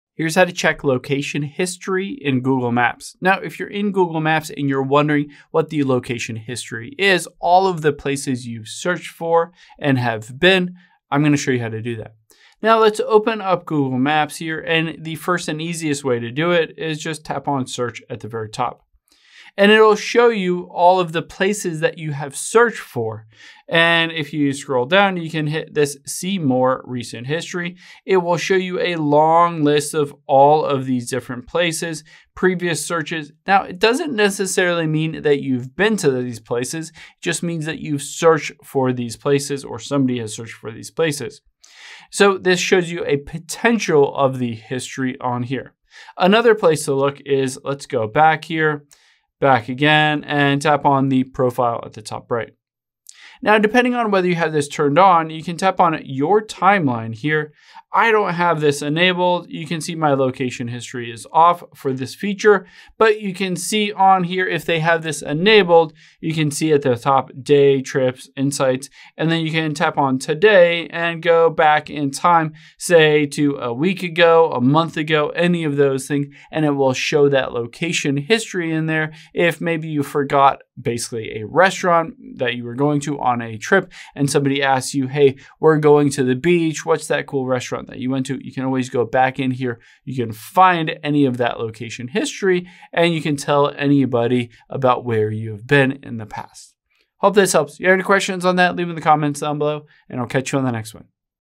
How To Check Location History In Google Maps
Here's how you can check your location history in Google Maps if you want to see where you visited in the past or where you navigated to.